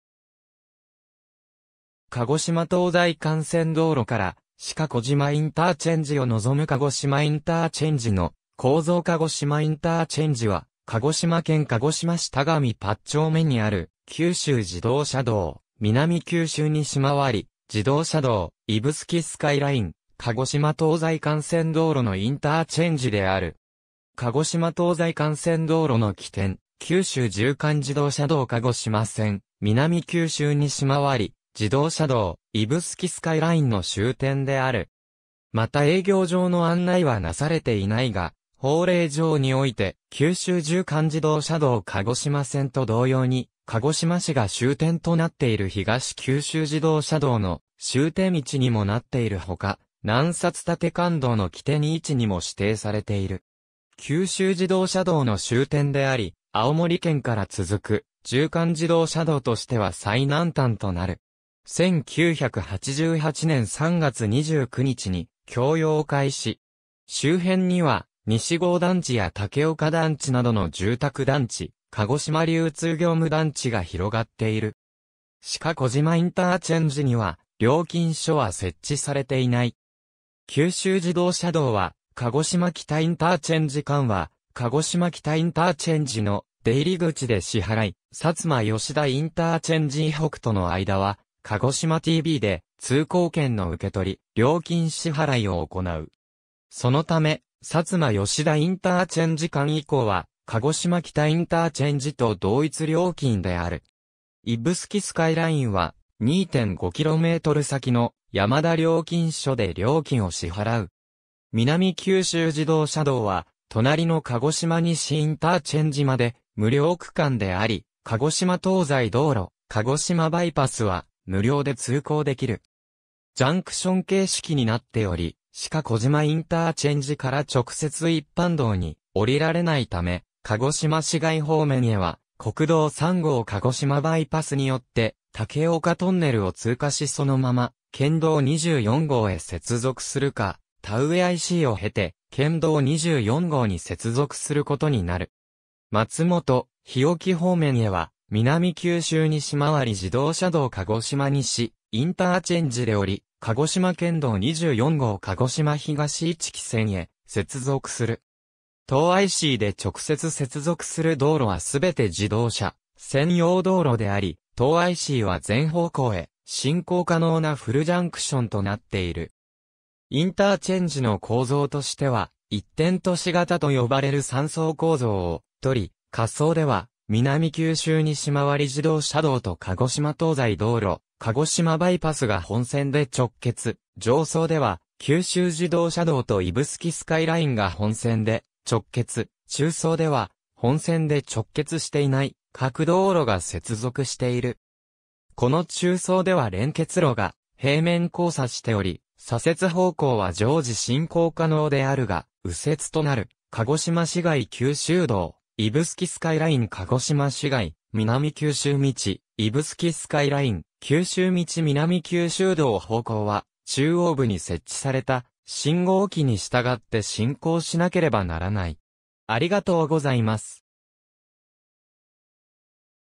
鹿児島インターチェンジ
鹿児島東西幹線道路（撮影当時未開通）から鹿児島ICを望む 鹿児島インターチェンジの構造 鹿児島インターチェンジ（かごしまインターチェンジ）は、鹿児島県鹿児島市田上八丁目にある九州自動車道・南九州西回り自動車道（鹿児島道路）・指宿スカイライン・鹿児島東西幹線道路のインターチェンジである。
鹿児島東西幹線道路（地域高規格道路）の起点、九州縦貫自動車道鹿児島線（高速自動車国道）・南九州西回り自動車道（一般国道自動車専用道路）・指宿スカイライン（鹿児島県道17号指宿鹿児島インター線、一般有料道路）の終点である。
また営業上の案内はなされていないが、法令上において九州縦貫自動車道鹿児島線と同様に鹿児島市が終点となっている東九州自動車道の終点位置にもなっているほか、南薩縦貫道（地域高規格道路）の起点位置にも指定されている。
九州自動車道の終点であり、青森県から続く縦貫自動車道としては最南端となる。
1988年3月29日に供用開始。
周辺には西郷団地や武岡団地などの住宅団地、鹿児島流通業務団地が広がっている。
鹿児島ICには料金所は設置されていない。
九州自動車道は、鹿児島北IC間は鹿児島北ICの出入口で支払い、薩摩吉田IC以北との間は鹿児島TBで通行券の受取・料金支払を行う。
そのため、薩摩吉田IC間以降は鹿児島北ICと同一料金である。
指宿スカイラインは、2.5km先の山田料金所で料金を支払う。
南九州自動車道（鹿児島道路）は隣の鹿児島西ICまで無料区間であり、鹿児島東西道路・鹿児島バイパスは無料で通行できる。
ジャンクション形式になっており、鹿児島ICから直接一般道に降りられないため、鹿児島市街方面へは国道3号鹿児島バイパスによって武岡トンネルを通過しそのまま県道24号へ接続するか、田上ICを経て県道24号に接続することになる。
松元・日置方面へは南九州西回り自動車道鹿児島西ICで降り、鹿児島県道24号鹿児島東市来線へ接続する。
当ICで直接接続する道路は全て自動車専用道路であり、当ICは全方向へ進行可能なフルジャンクションとなっている。
インターチェンジの構造としては、一点都市型と呼ばれる3層構造をとり、下層では南九州西回り自動車道と鹿児島東西道路・鹿児島バイパス（鹿児島市街方面）が本線で直結、上層では九州自動車道と指宿スカイラインが本線で直結、中層では本線で直結していない各道路が接続している。
この中層では連結路が平面交差しており、左折方向は常時進行可能であるが、右折となる鹿児島市街→九州道、指宿スカイライン→鹿児島市街、南九州道→指宿スカイライン、九州道→南九州道方向は中央部に設置された信号機に従って進行しなければならない。